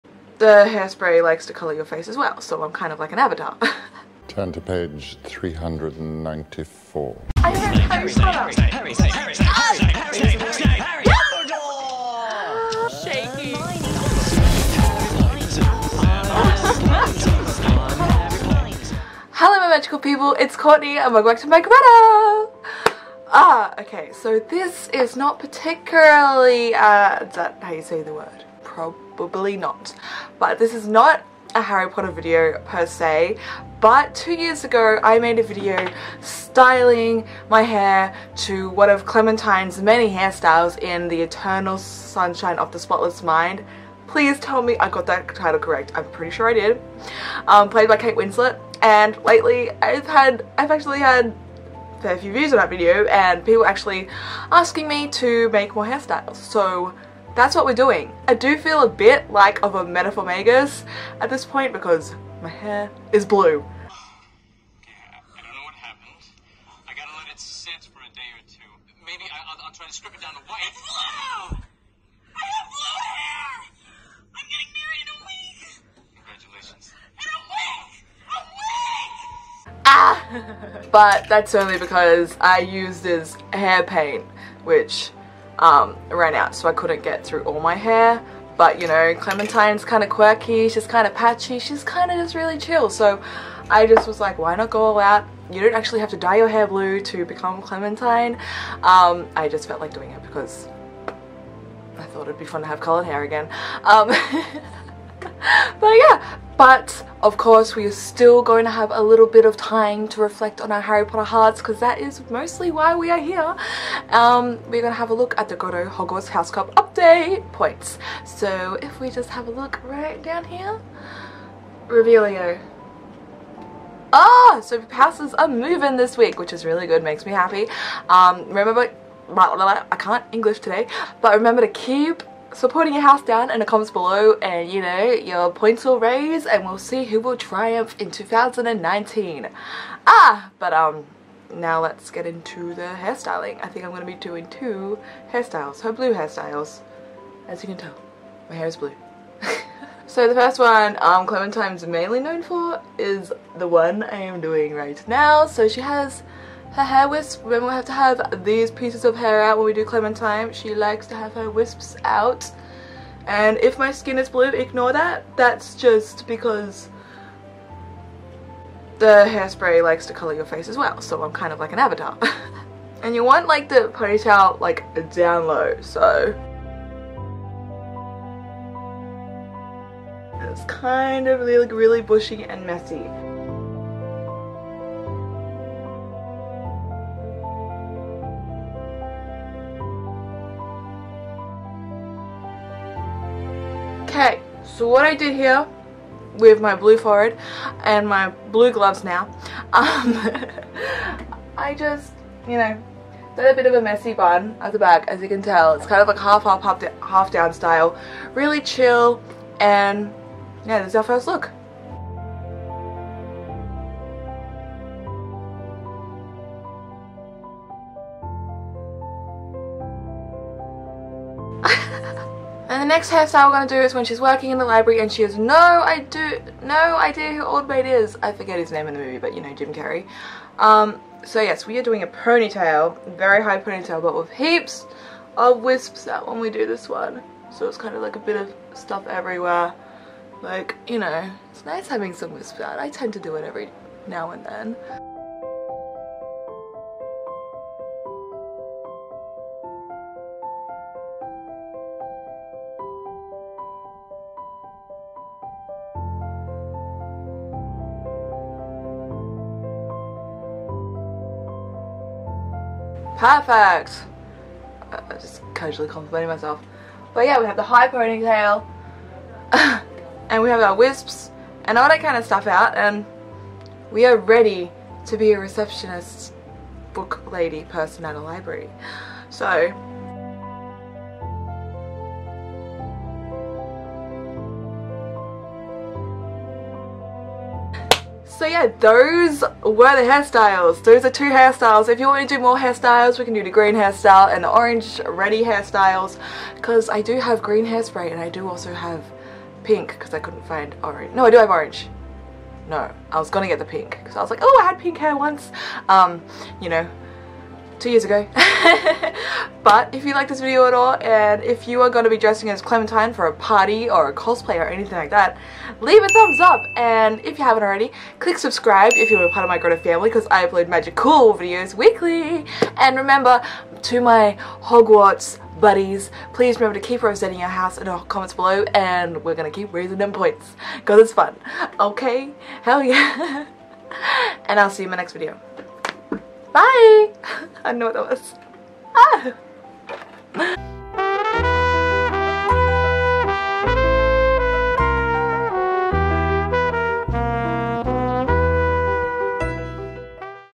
Kate Winslet Inspired Hair Tutorial - Eternal Sunshine of the Spotless Mind (2)
Something not so Potter-ish today!
I had some people ask me to do another Clementine look from the film Eternal Sunshine of the Spotless Mind from a previous video so here it is!

If you’re reading this make sure to tell me your Hogwarts House in the comment section and you might be rewarded with some house points ;)